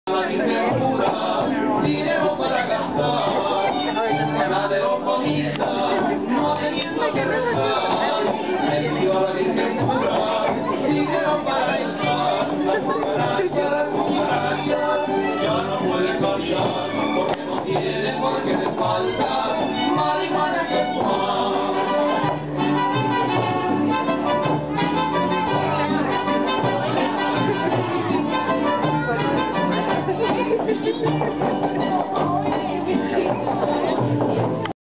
Cinco de Mayo
Program held at Ellenville Public Library & Museum Community Room, followed by parade to parking lot by Gaby's Restaurant.